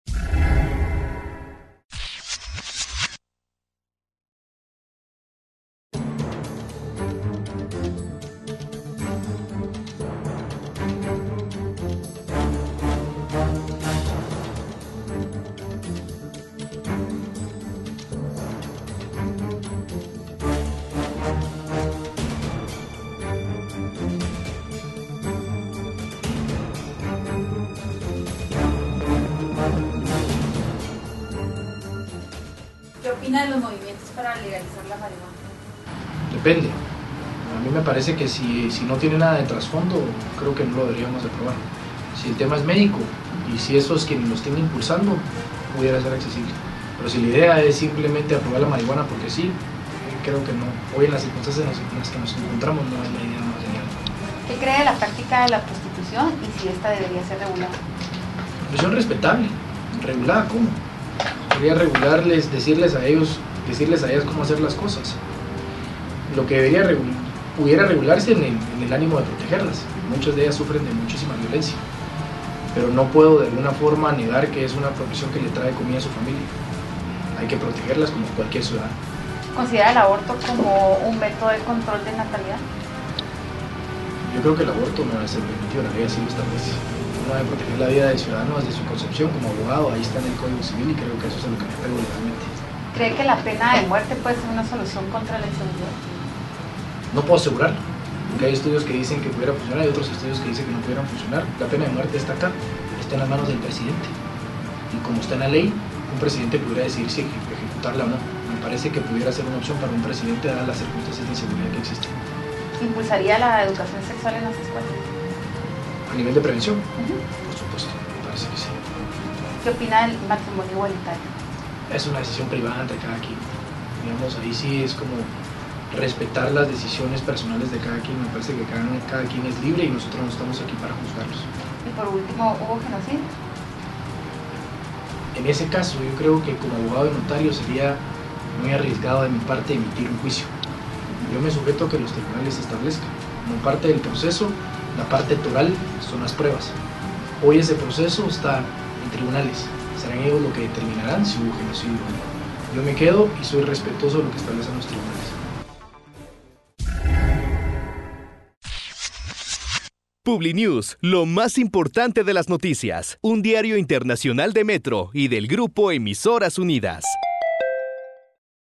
"Veneno" Cruz atraviesa por el "Campo Minado"
Publinews realizó el segmento "Campo Minado" en el que somete a los candidatos a los diferentes puestos de elección popular, a una serie de temas polémicos y de interés para la sociedad, con el objetivo de conocer a grandes rasgos su perspectiva sobre aspectos que los políticos prefieren evitar. En esta ocasión, el turno fue para el candidato a la alcaldía capitalina por el Partido Visión con Valores, Luis Enrique "Veneno" Cruz

Por: Luis Carlos Nájera / Oliver de Ros